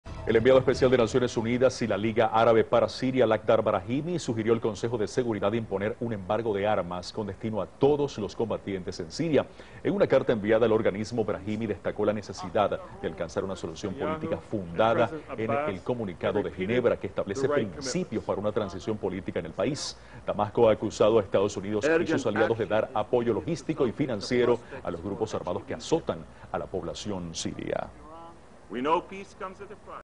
Brahimi propõe embargo de armas na Síria
O enviado especial da ONU e da Liga Árabe para a Síria, Lakhdar Brahimi, propôs ao Conselho de Segurança um embargo de armas a todos os envolvidos nos enfrentamentos no país árabe. teleSUR